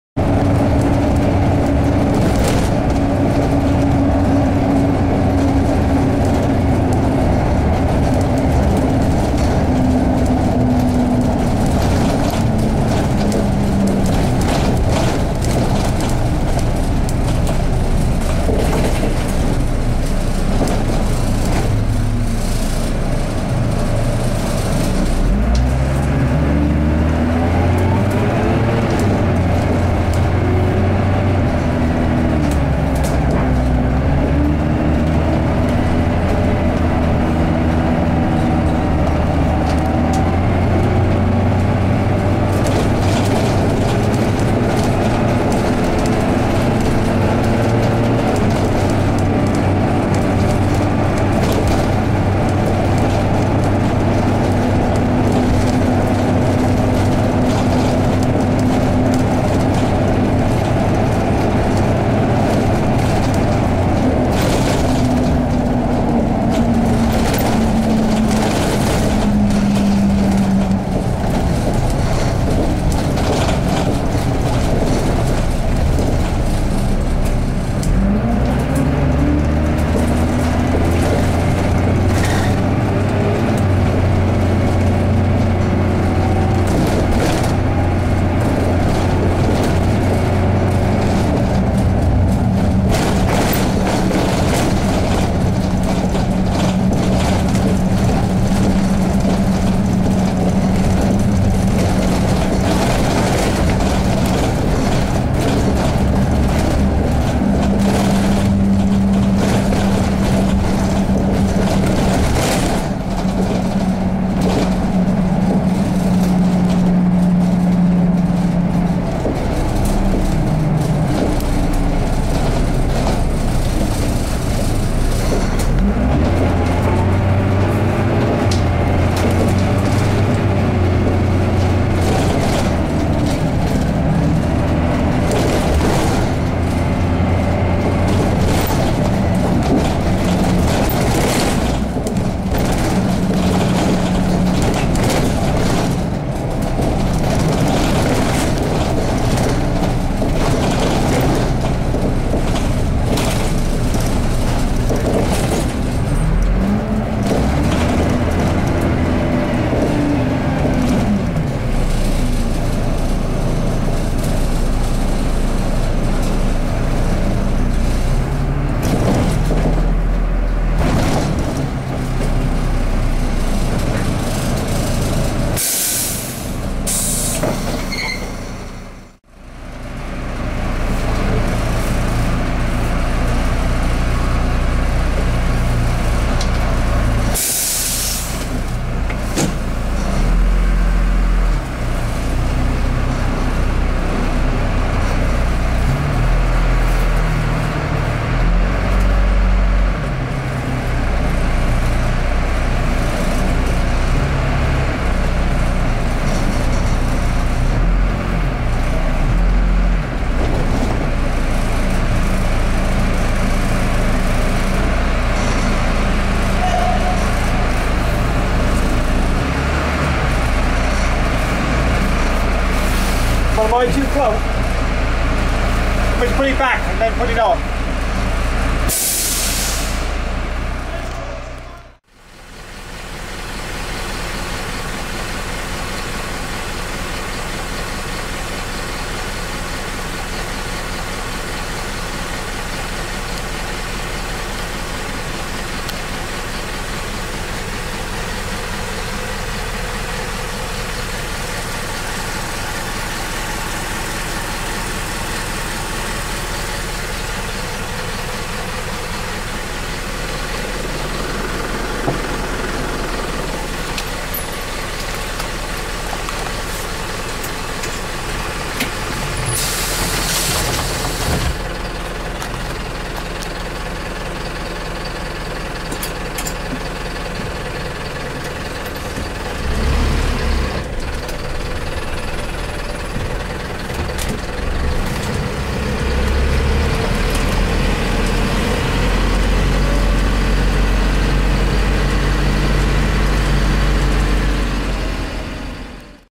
Wellington Last
The Time i took a bus to the old ex Midland Red Garage at Wellington on the 13th April 2012. Video from SLF Dart 2248 (W248SNR) .